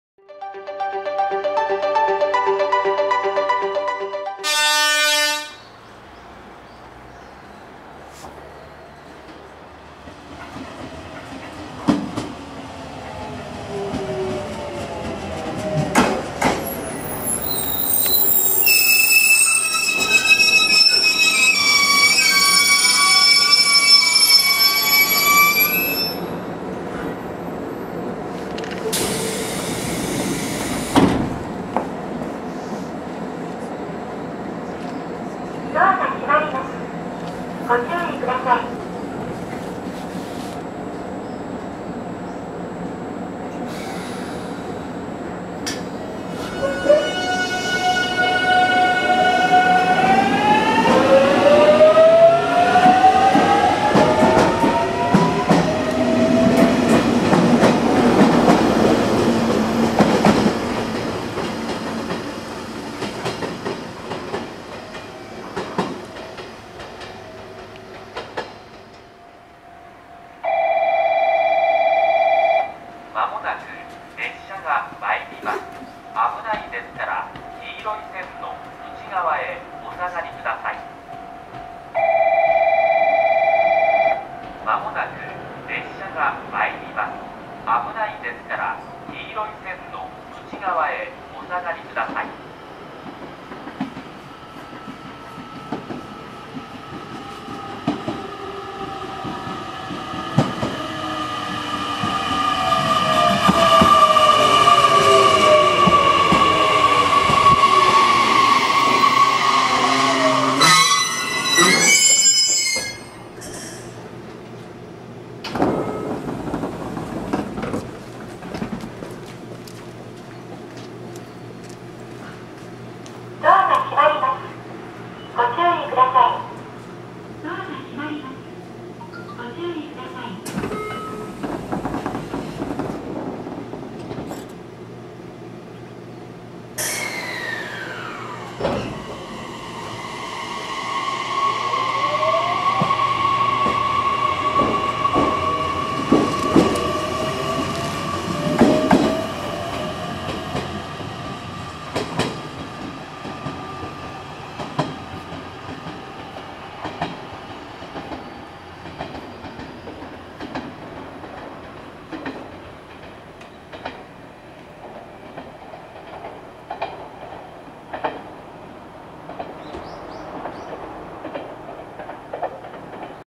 【鉄道】JR九州 福北ゆたか線817系 in 天道駅【train】
飯塚市内にある福北ゆたか線の天道駅にて博多行きと直方行きの電車を撮影しました。